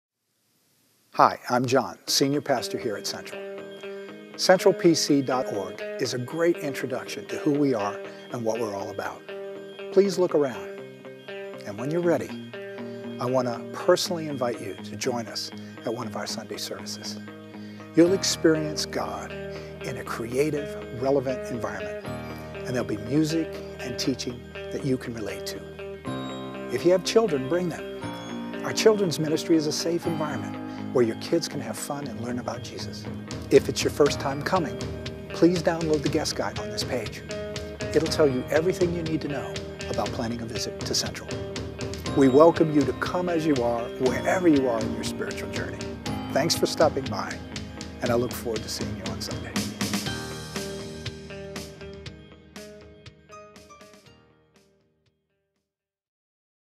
Central Presbyterian Church in Towson MD Web Intro
A welcome from the Senior Pastor of Central Presbyterian Church located in Towson, Maryland.